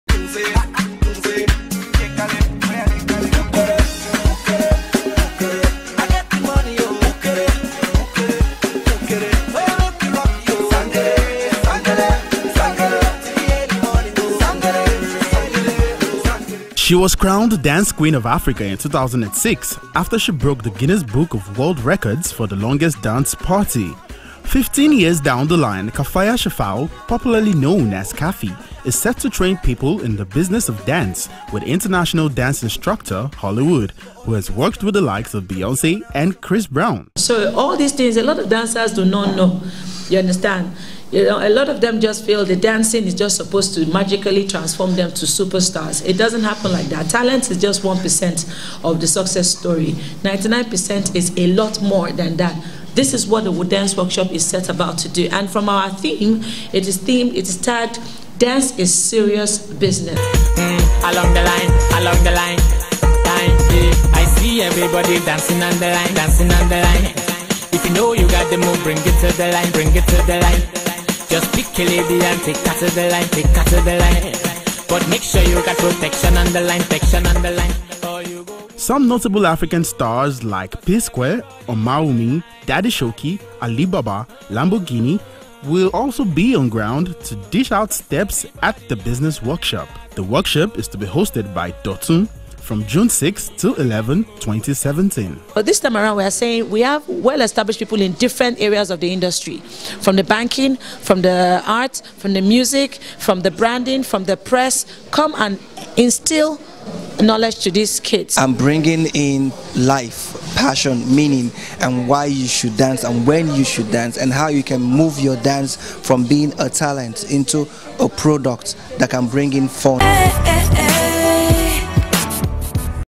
KAFFY SPEAKS ON DANCE BUSINESS
She was crowned the dance queen of Africa in 2006 after she broke the Guinness book of world records for the longest dance party.

15 years down the line Kafaya Shafau popularly known as Kaffy is set to train people in the business of dance with international dance instructor of Hollywood who has worked with the likes of Beyonce and Chris Brown.